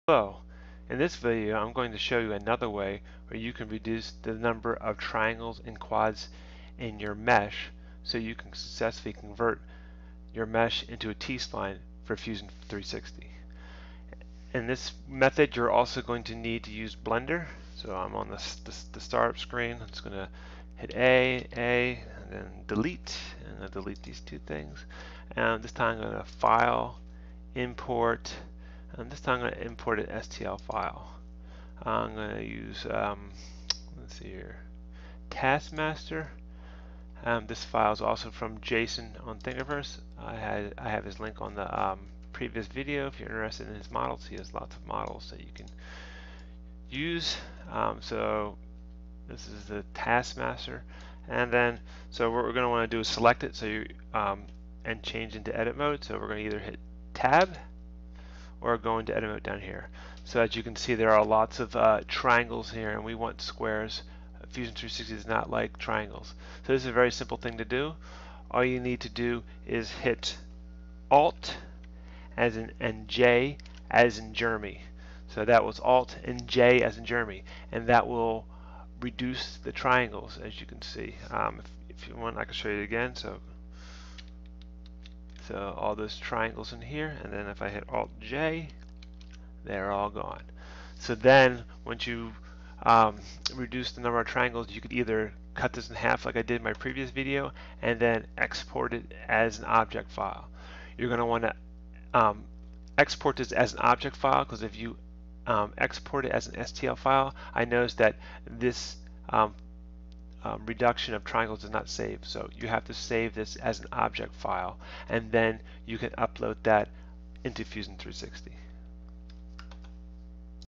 How to reduce the number of triangles and quads for Fusion 360 (Part 2)
This video will discuss how reduce the number of triangles and quads to be able to convert your mesh into a t-spline for fusion 360.